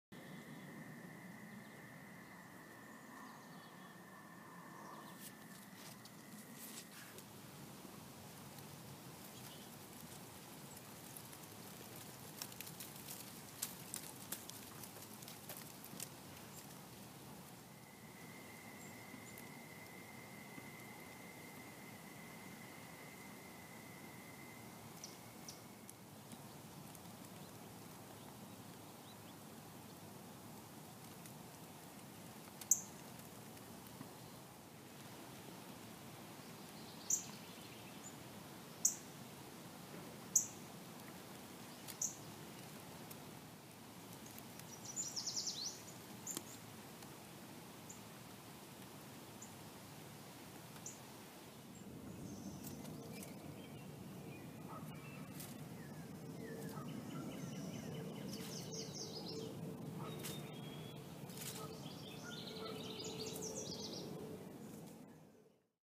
Cardinal Nest with 6 Speckled Eggs (Happy Mother's Day) - Quiet Moments
One of the six speckled Cardinal eggs looks like a smiley face :)